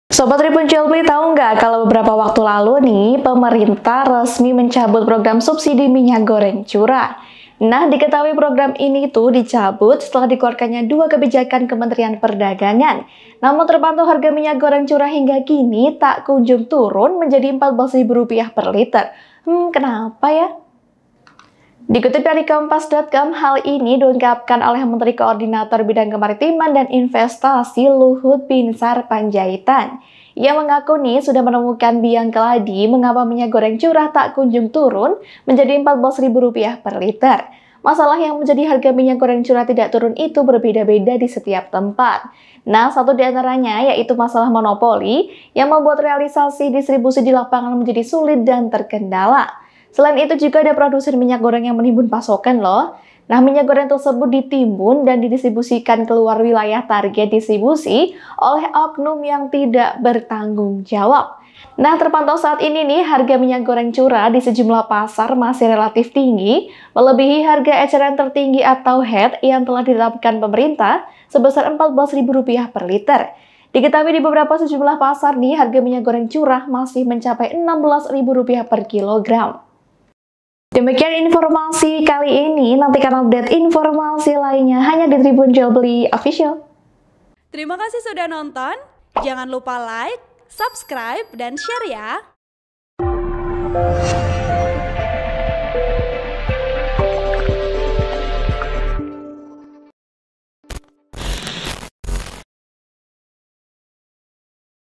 Ini Biang Kerok Harga Minyak Goreng Curah Tak Kunjung Turun Jadi Rp 14 Ribu, Kini Dibanderol Segini
TRIBUN-VIDEO.COM - Beberapa waktu lalu pemerintah resmi mencabut program subsidi minyak goreng curah.

Diketahui, program ini dicabut setelah dikeluarkannya dua kebijakan Kementerian Perdagangan.

Namun, terpantau harga minyak goreng curah hingga kini tak kunjung turun menjadi Rp 14.000 per liter.

Artikel ini telah tayang di Kompas.com dengan judul "Kata Luhut, Ini Biang Kerok Harga Minyak Goreng Curah Tak Turun Jadi Rp 14.000"